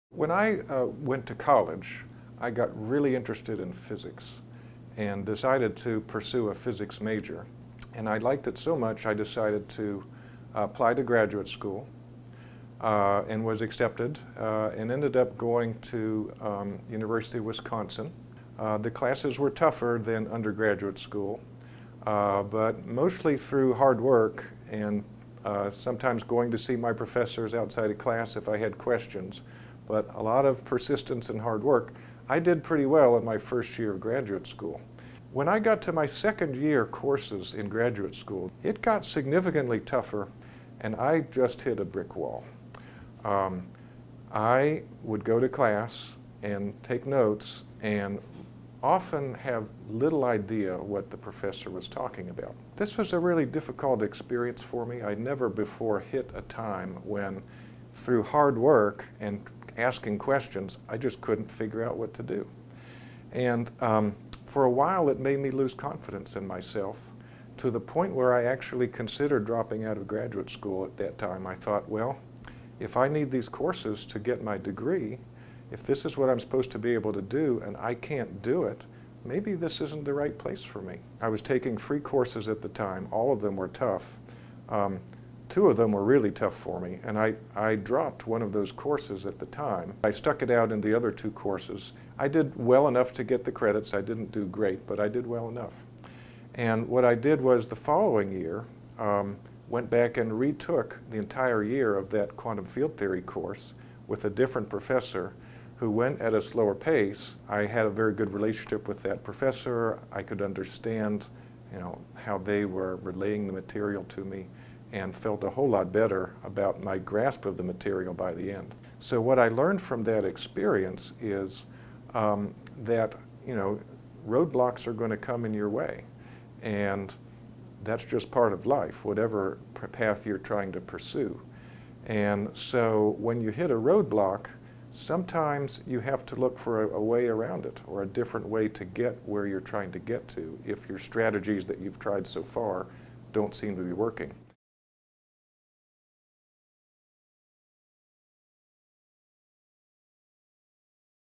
The NCSSM Resilience Project: Jon Bennett
Failure, struggle and faltering are normal components of our lives that can provide growth, insight, and unforeseen opportunities.

During the 2017-18 school year, NCSSM Counseling Services partnered with faculty and staff to create the Resiliency Project. This initiative showcases the tremendous growth and strength that can come from moments of difficulty throughout our lives, whether academic or personal in nature.

A heartfelt thanks to Carrie Alter, Christine Belledin, Jon Bennett, Michelle Brenner, Laura Baker Campbell, Bob Gotwals, Kyle Hudson, Alina Hunt, Terry Lynch, Adam Sampieri, and Todd Roberts for sharing their stories with us. It takes strength, vulnerability and a desire to make a difference in the world to share your most challenging times with others. Also, a big thank you to Joyce Ventimiglia and Sam Borkovic who graciously spent time recording and editing the recordings to create the final product. We are deeply grateful to all of you!